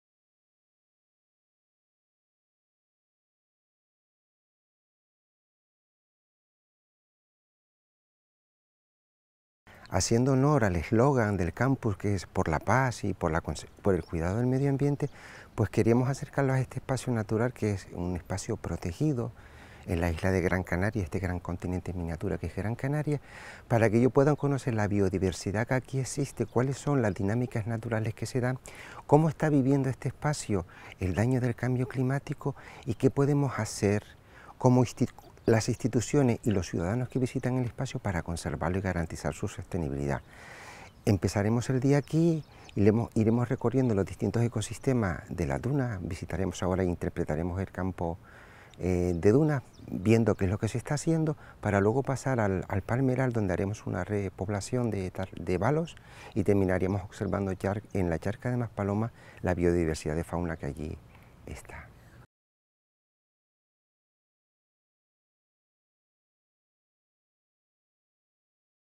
IX CAMP Internacional Rotary Maspalomas - Resumen entrevistas 04 jul 2023_02
Biodiversidad y prácticas sostenibles en el CAMP.
Los asistentes al IX CAMP Internacional Rotary Maspalomas han visitado la Reserva Natural de las Dunas de Maspalomas y han aprendido sobre el funcionamiento del Centro Espacial de Canarias